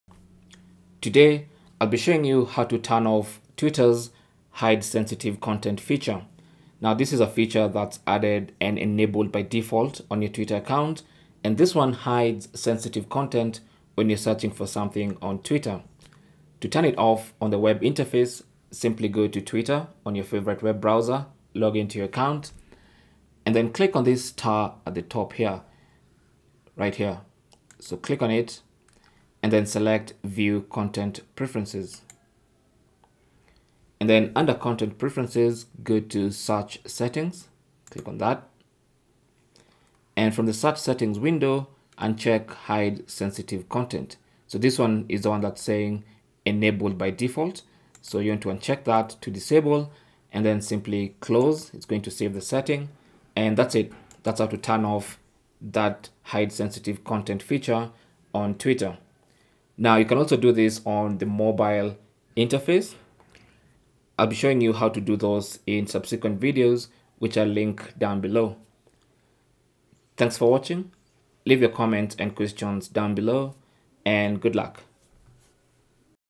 How To Turn Off 'Hide Sensitive Content' On Twitter (Web)
How to disable the hide sensitive content feature on Twitter on the web interface.

Log in to Twitter
Click on the start at the top right
Select 'Content Preferences'
Uncheck 'Hide Sensitive Content'

Cell Phone Tripod Adapter